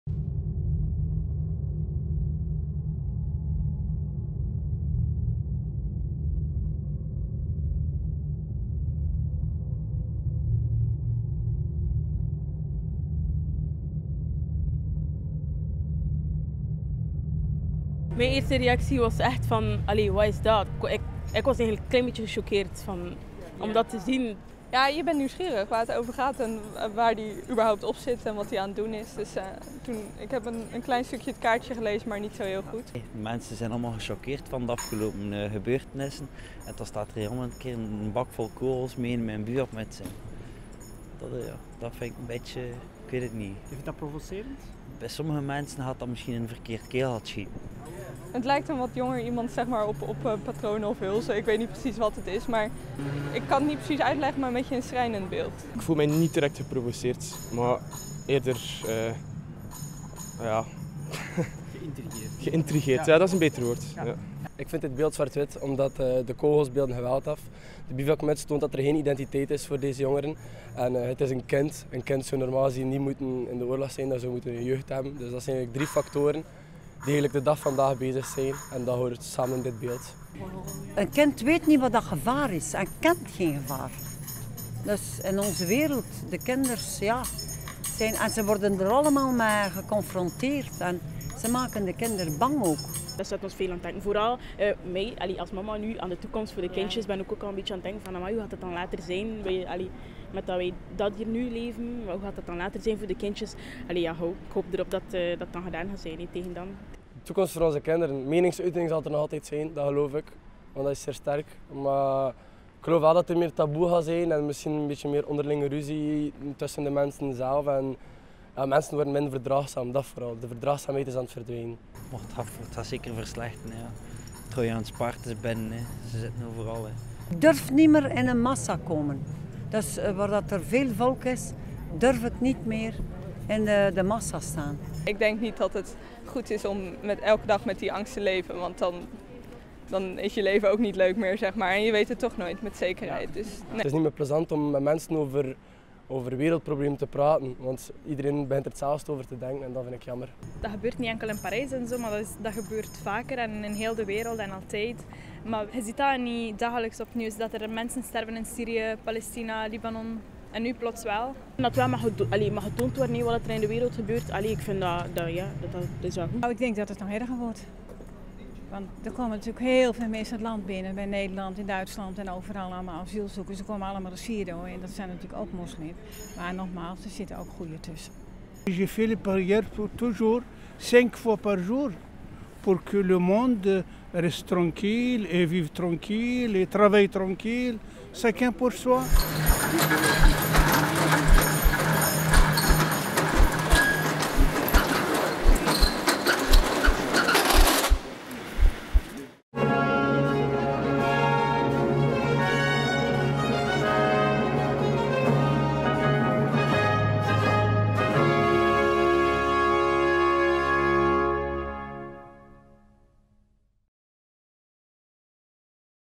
Ceci n'est pas Kortrijk #4 - Ceci n'est pas le futur
Is een gemaskerd kind op een stapel kogels een relevante verbeelding van de aanslagen in Parijs, is het een zinloze provocatie of heeft het hoegenaamd niets te maken met de recente terreurdaden? Wordt de toekomst beter of slechter? En in wat voor wereld gaan onze kinderen opgroeien?

We stelden deze vragen aan het Kortrijkse publiek over de installatie Ceci n'est pas le futur, het vierde beeld uit de reeks Ceci n'est pas Kortrijk van Dries Verhoeven die in het kader van het NEXT-festival in de Kortrijkse winkelstraten wordt getoond.

Volg het hele project mee op en deel jouw mening, foto's en filmpjes via cecinestpaskortrijk!

De Zendelingen is een multimediaal kunstkritiekcollectief dat meerstemmige meningen over podiumkunst capteert en ontsluit.